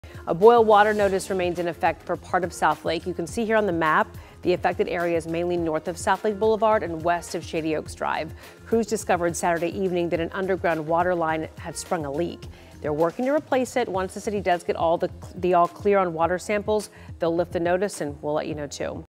Boil water notice remains in effect for parts of Southlake, Texas
Officials said a water line near Southlake Blvd and Randol Mill developed a large leak.